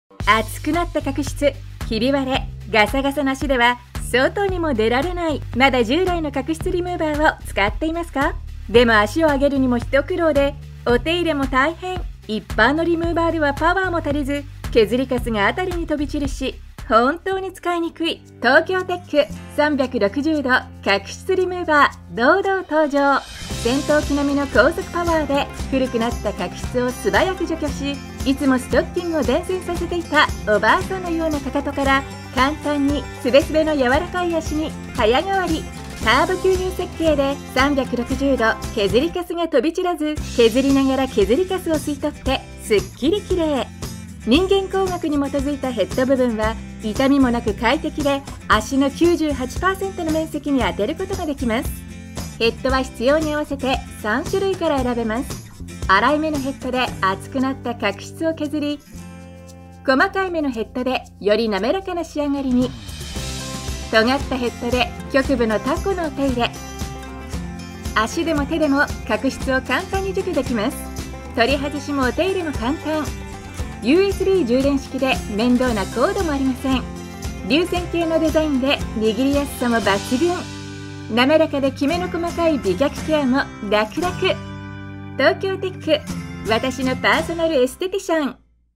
Tokyotek Electric Foot Callus Remover
Product Features:

●Arc Shape Grinding Head: With the ergonomic grinding head, the machine can touch 98% of your skin while treating hard thick callus

●Powerful Motor: The speed of the motor can reach up to 14,000 RPM. The machine can easily buff your dry dead skin

●3 Different Quartz Grinding Heads: The machine comes with three different grinding heads for options so you can choose the head according to your needs

●Suction Dust Function: With the turbin design, the dust and, dead skin and callus can be absorbed  and collected

●USB Charging: The machine can be charged through a power bank, laptop, and USB devices. It is very convenient to carry everywhere